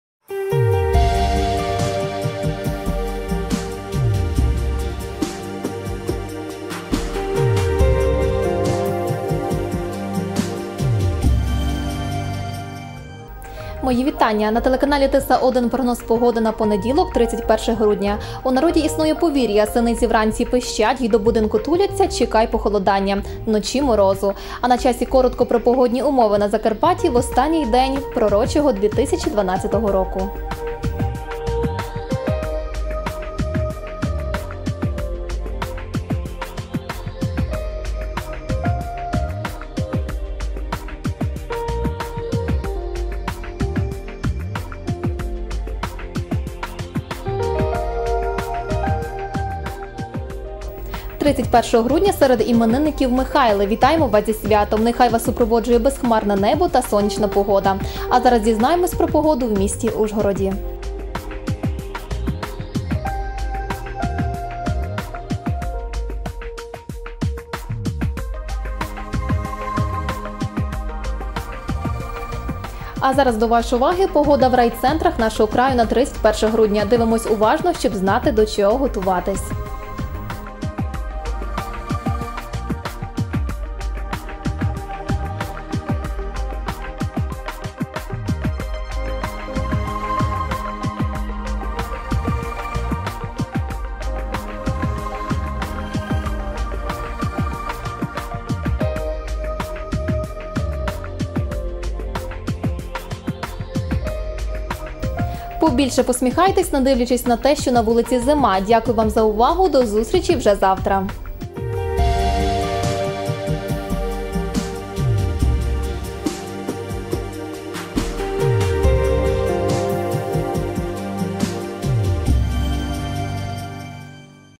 Тиса-1.Прогноз погоди 31 грудня.
От і дожили ми до останнього, 366-го, дня у році, що минає. Подібне будемо мати аж через чотири роки. А поки що подякуємо 2012-у за гарну загалом погоду для закарпатців. Чого ж чекати від останнього дня знає Ольга БЕЖЕВЕЦЬ.